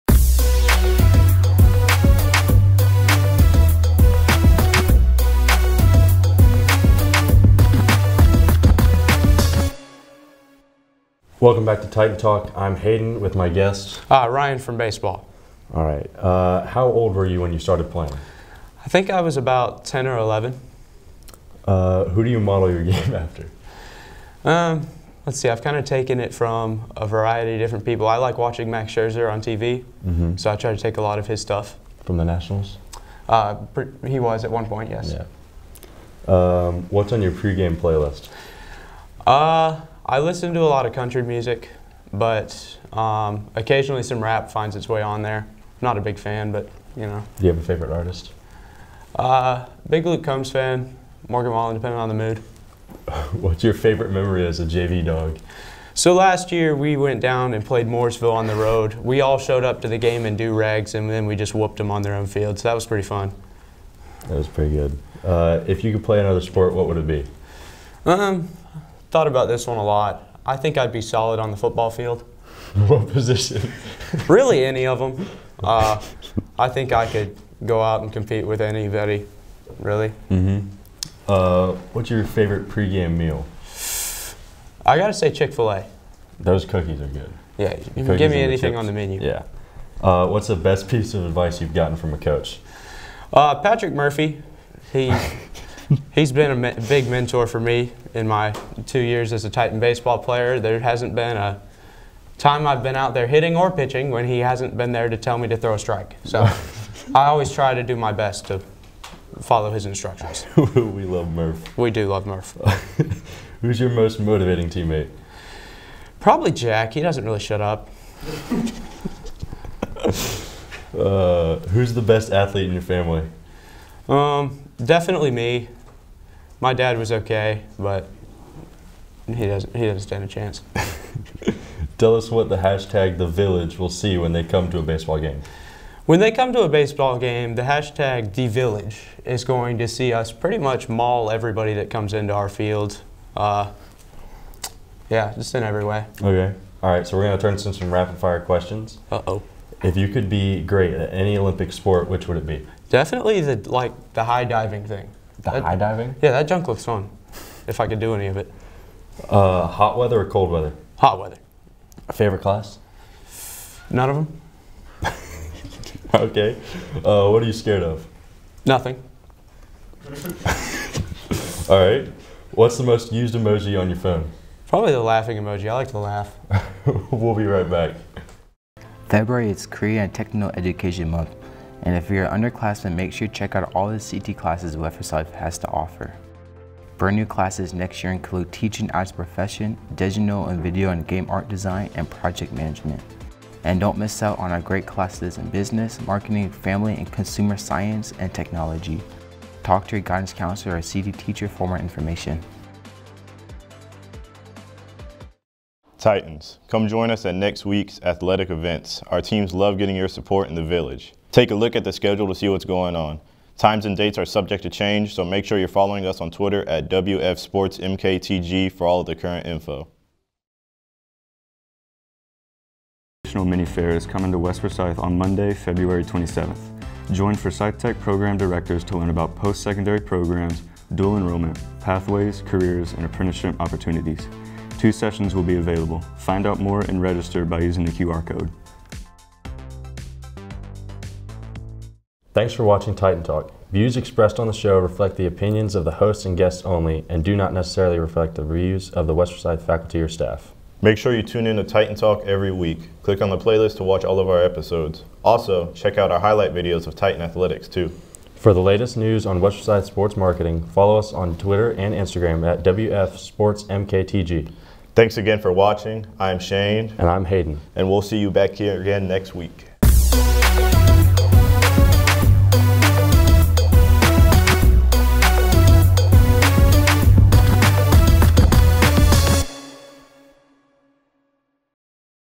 Titan Talk 3-24
Hayden talks to Ryan from the baseball team, and Shane runs down next week's home athletic schedule.